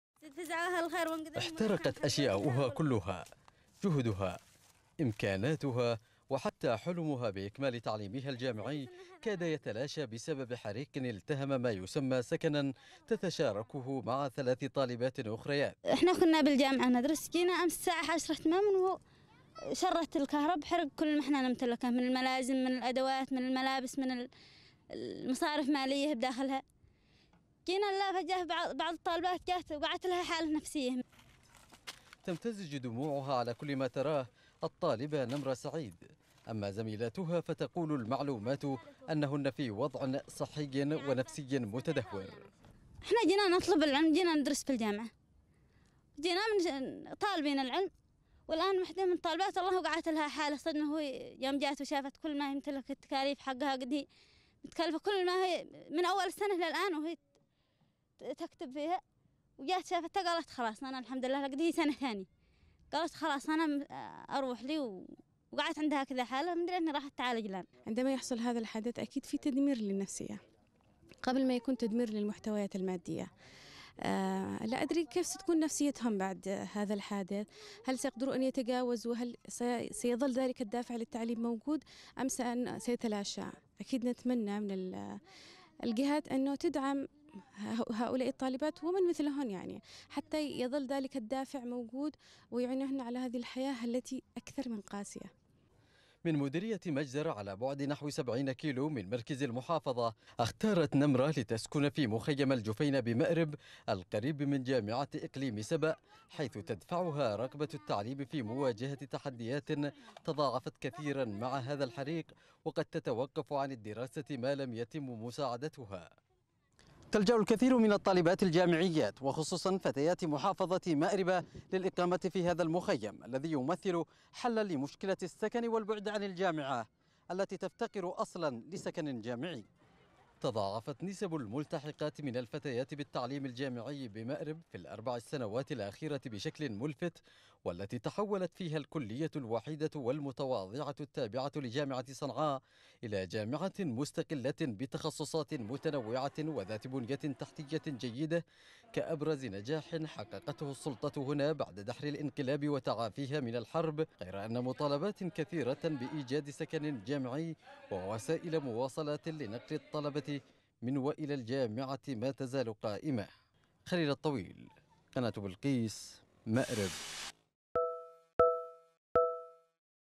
نسبة الملتحقات من الفتيات بالتعليم الجامعي في مأرب تضاعفت خلال السنوات 4 الأخيرة| تقرير: خليل الطويل
تضاعفت نسبة الملتحقات من الفتيات بالتعليم الجامعي في مأرب خلال السنوات الأخيرة.
ويعود ذلك إلى تحويل الكلية الوحيدة إلى جامعة مستقلة بعد أن كانت ملحقة بجامعة صنعاء.
وتمثل الطالبة نمرة سعيد التي تدرس في جامعة إقليم سبأ نموذجاً للفتيات في مأرب، إذ تتمسك بالحصول على حقها من التعليم رغم مواجهتها لتحديات وصعوبات كثيرة...